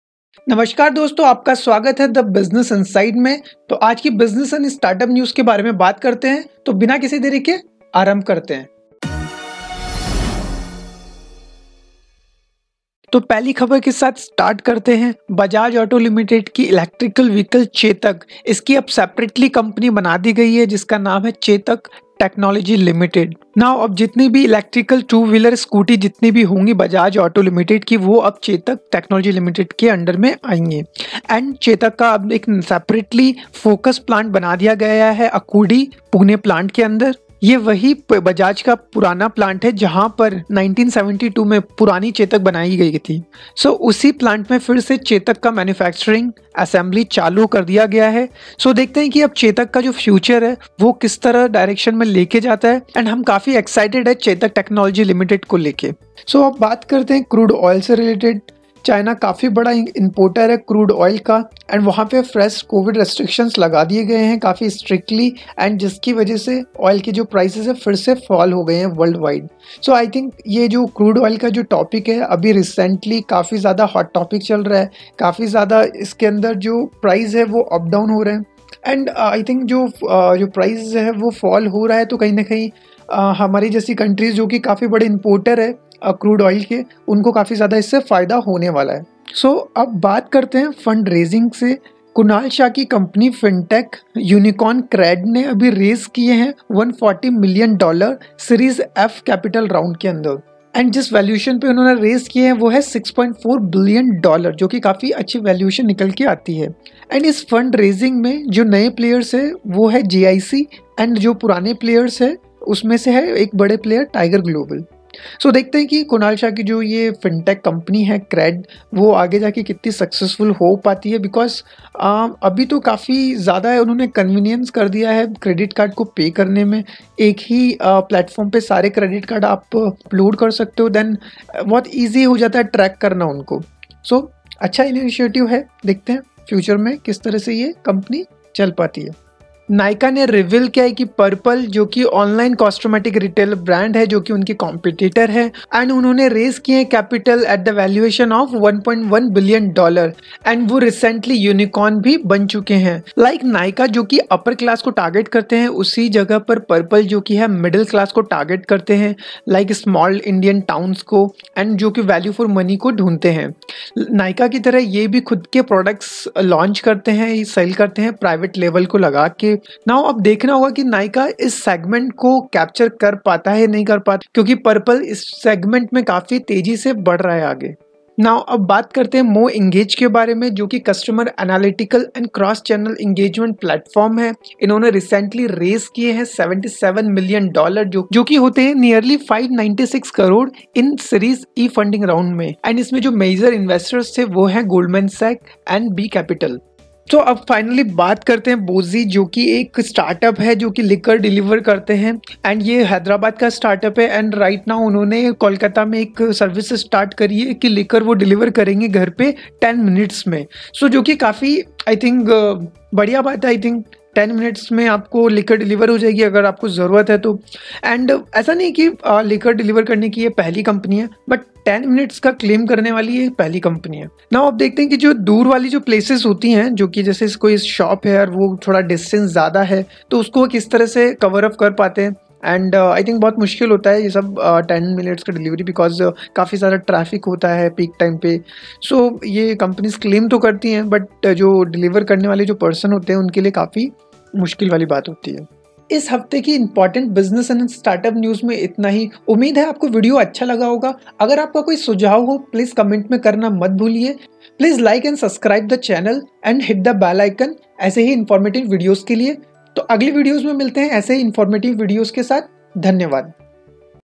News #39 | Rajiv Bajaj | Bajaj Auto | Chetak | Cred| Crude prices| Purplle| Nykaa| MoEngage| Booozie
||This week in Indian Startup News||

00:12  Rajiv Bajaj inaugurated Bajaj Auto's wholly-owned subsidiary Chetak Technology Ltd's
00:42  Oil prices fall as China imposes fresh covid restrictions
01:13 Cred raising Series F capital at $6.4 billion
01:37  Nykaa rival Purplle becomes India’s newest unicorn
01:37  MoEngage raises $77 mn from Goldman Sachs and B Capital
01:37  Now get liquor delivered in 10 minutes

Follow The Business inside news to remain updated with latest developments of business world.